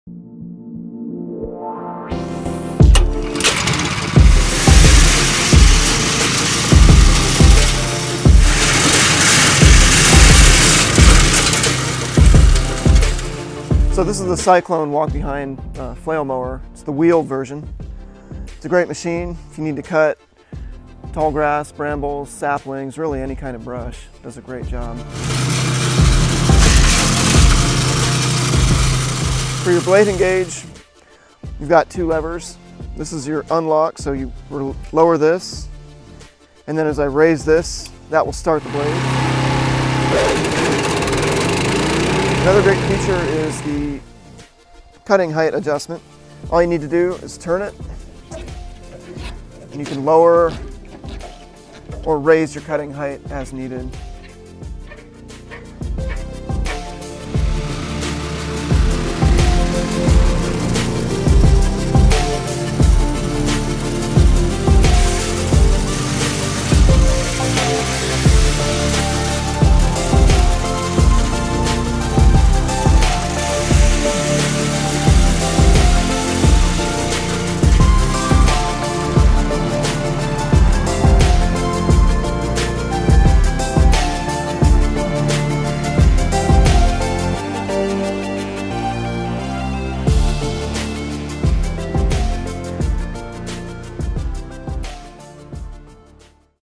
When a Cyclone Flail Mower Faces Some Scotch Broom
Our Cyclone walk-behind flail mower loves to mulch down tall grass and brush. Watch as this wheeled version of the Cyclone takes down a field of hearty scotch broom. Outdoor professionals become quickly speechless when they witness how easily the Cyclone sucks in the brush and mulches it up with its system of 38 flail blades.